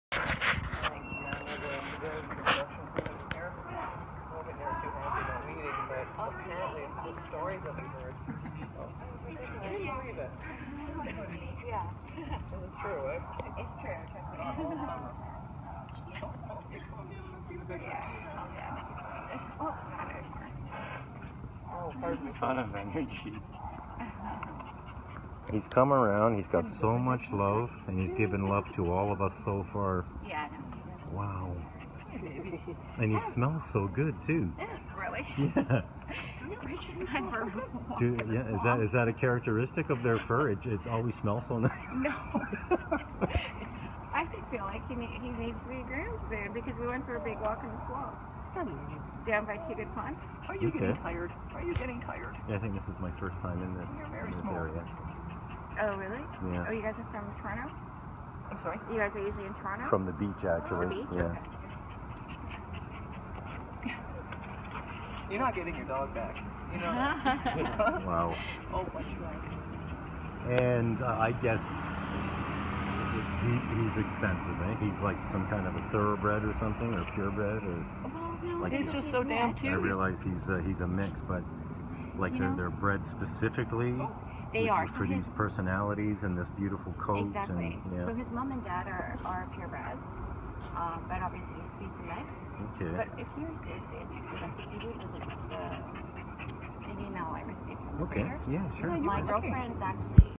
Micky- The Most Beautiful Little Dog I've Ever Seen
Hey guys.  I'm part of a landscaping team, for the short time I'm Toronto, and we came across Irene, who was walking her lovely dog, Mickey.  We were all gaga over this little guy.  He's a mix of Toy Poodle and Bolonoodle.  Turns out that Bethany Miller, at Jarbeth Kennels, has puppies for sale.  You can reach her at